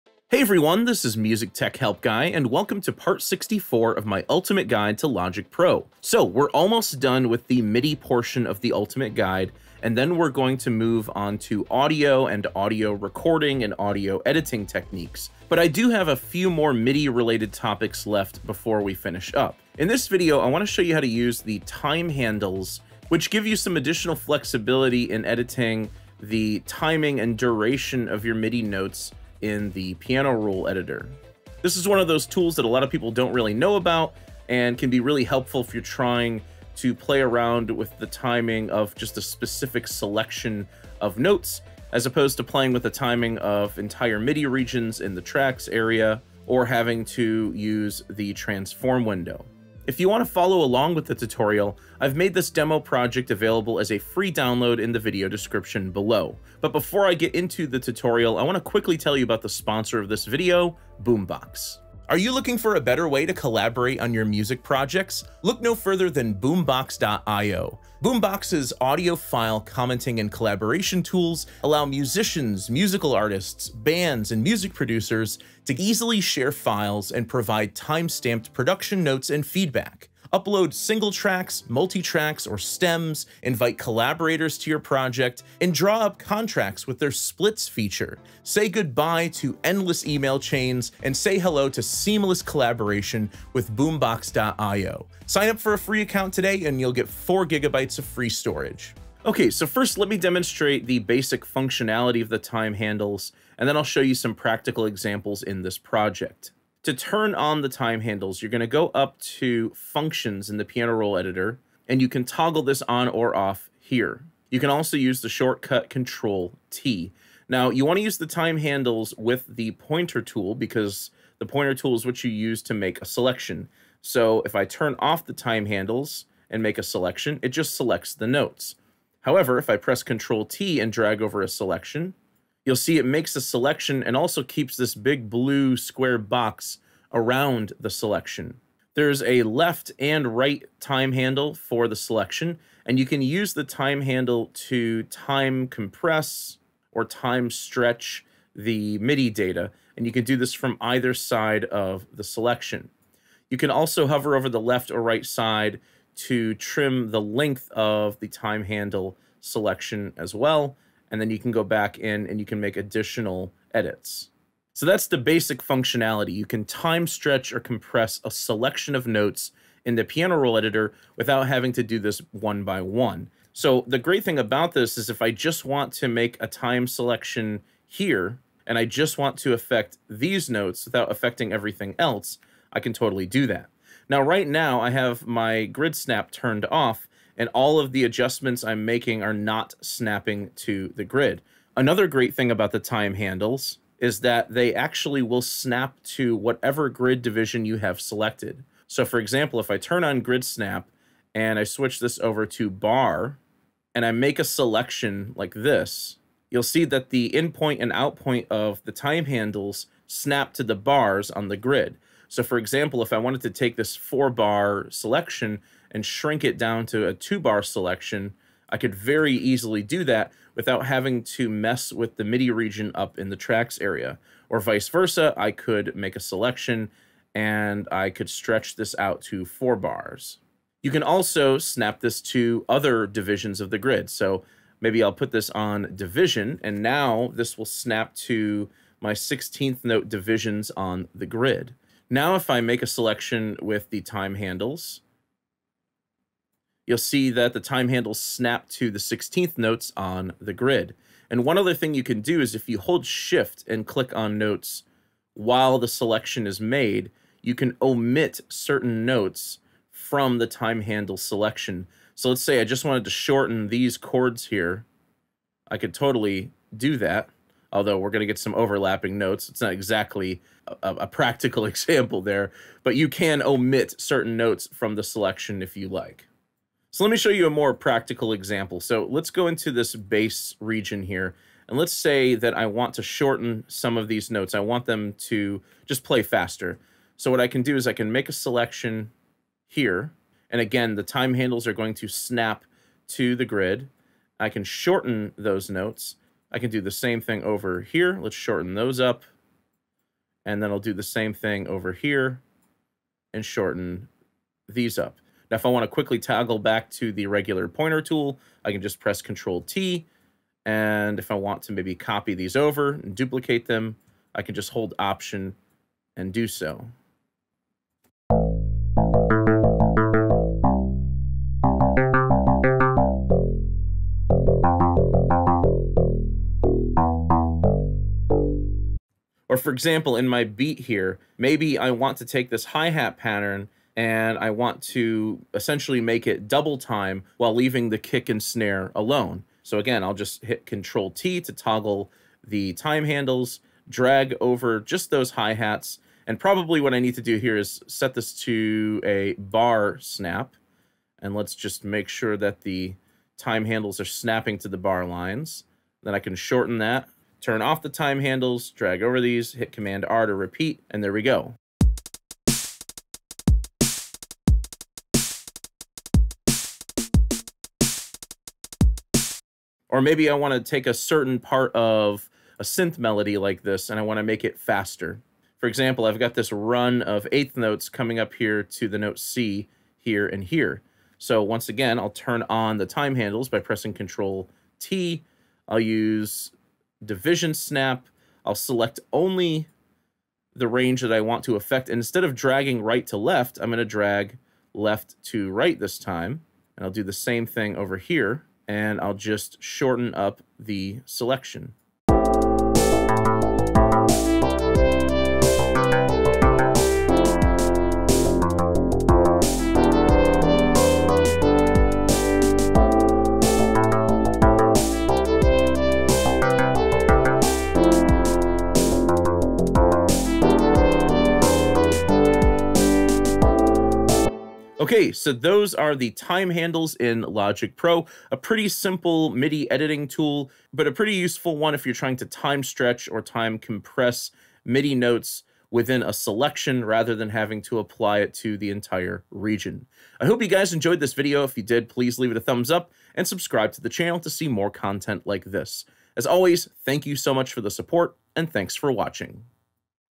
Logic Pro #64 - Time Handles (Time Stretch MIDI Notes!)
In this video, I demonstrate the following:
1. How to access the time handles in the piano roll editor.
2. How to use the time handles to time stretch or compress MIDI notes.
3. How to use the time handles with grid snap.

0:00 Introduction
1:01 Sponsor Segment
1:39 Basic Functions
3:04 Grid Snap
4:38 Practical Examples